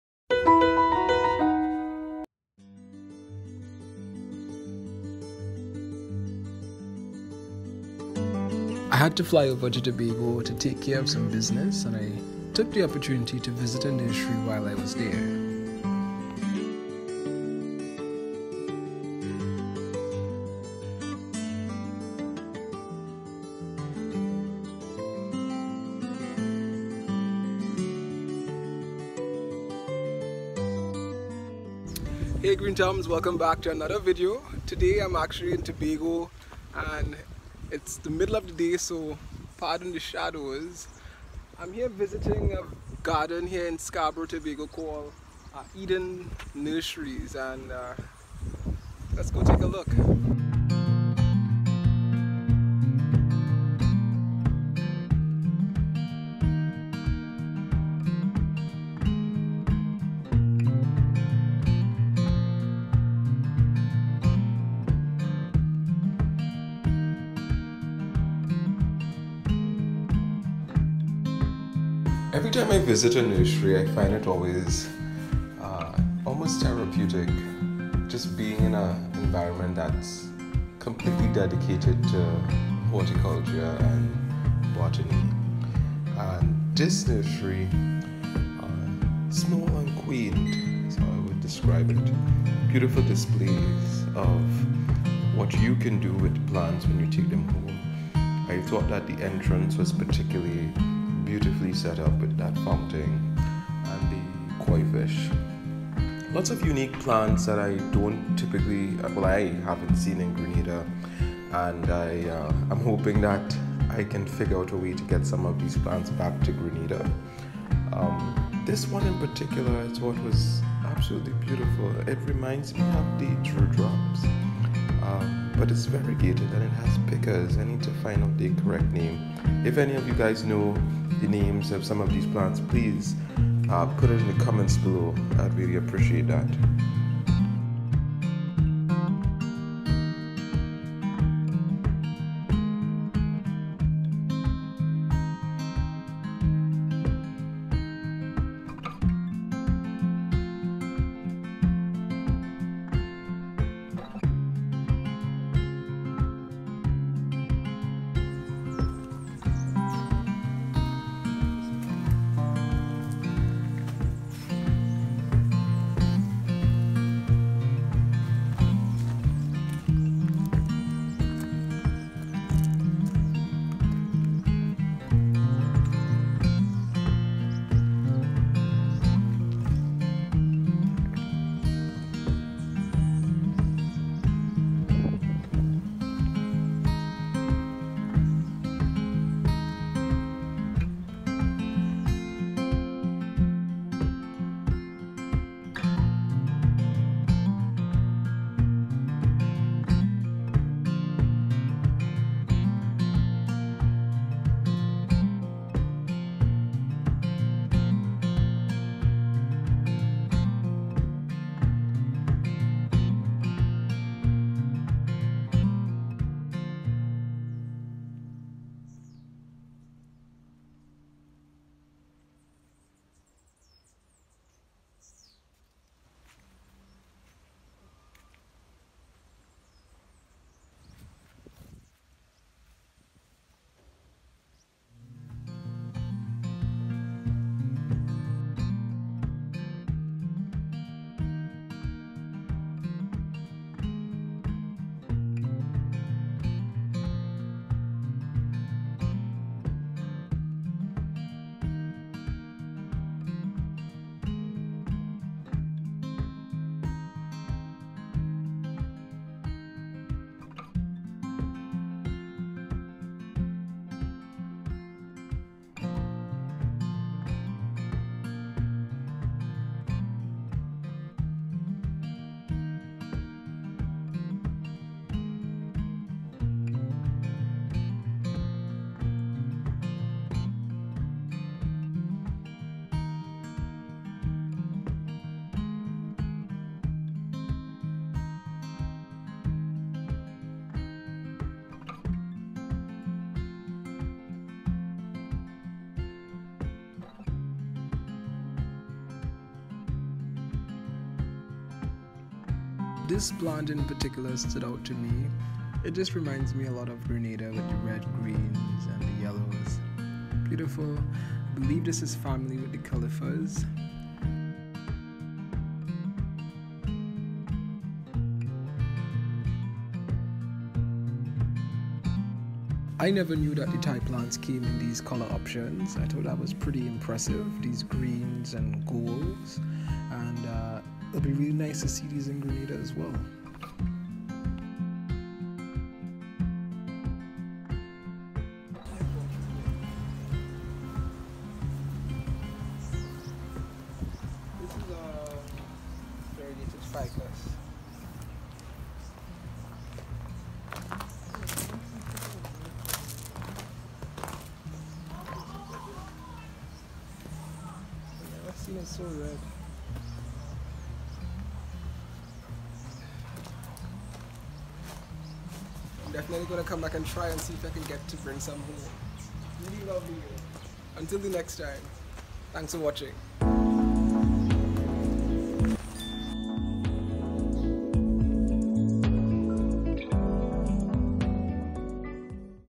Eden’s Nursery | Tobago Nursery Visit | Green Thumbs
I had to fly home to Tobago for a few days and was blessed to have to chance to visit this small nursery in Scarborough. It was full of charm and unique plants I hope to add to my garden. Happy planting